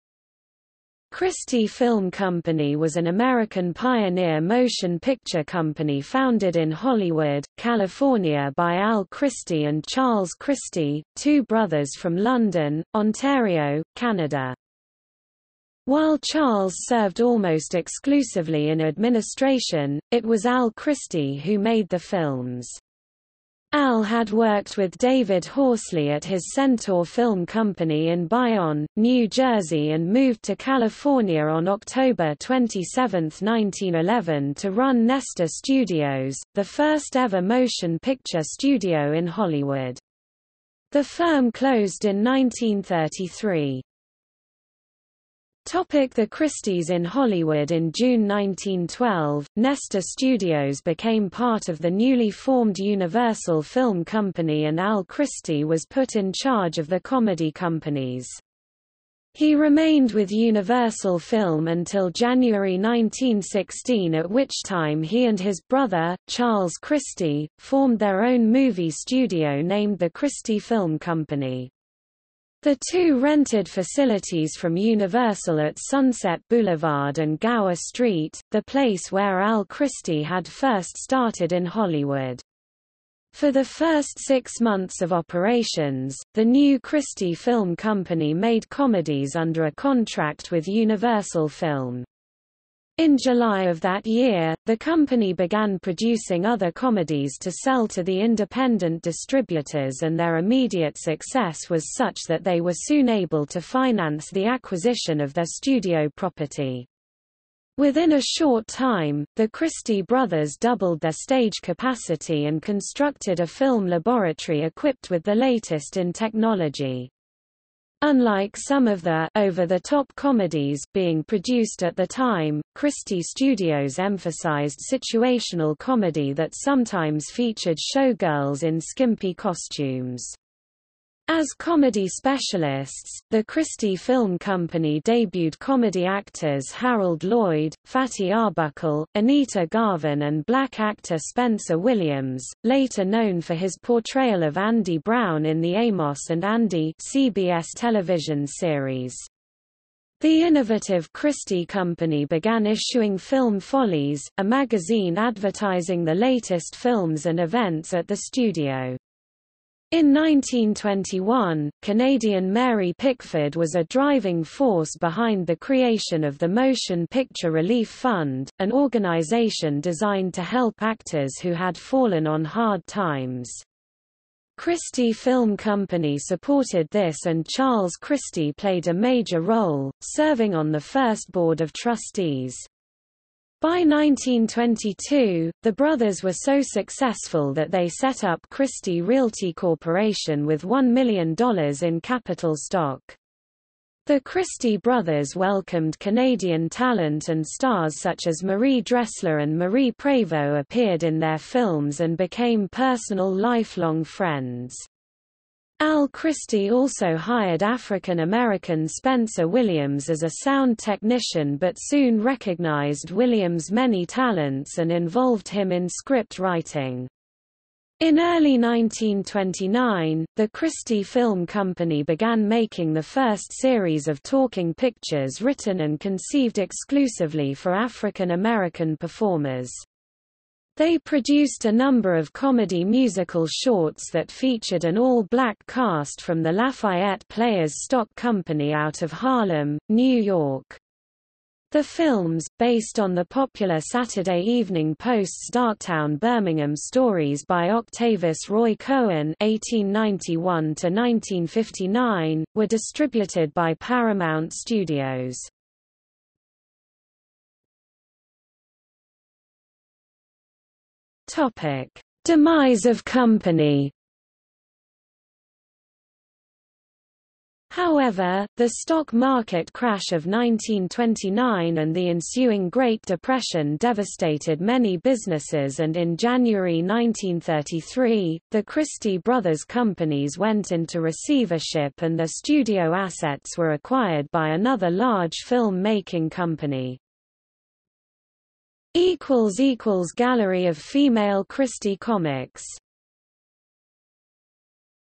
Christie Film Company | Wikipedia audio article
00:00:45 1 The Christies in Hollywood
00:04:17 2 Demise of company

- Socrates

SUMMARY
Christie Film Company was an American  pioneer motion picture company founded in Hollywood, California by Al Christie and Charles Christie, two brothers from London, Ontario, Canada.
While Charles served almost exclusively in administration, it was Al Christie who made the films. Al had worked with David Horsley at his Centaur Film Company in Bayonne, New Jersey and moved to California on October 27, 1911 to run Nestor Studios, the first ever motion picture studio in Hollywood.  The firm closed in 1933.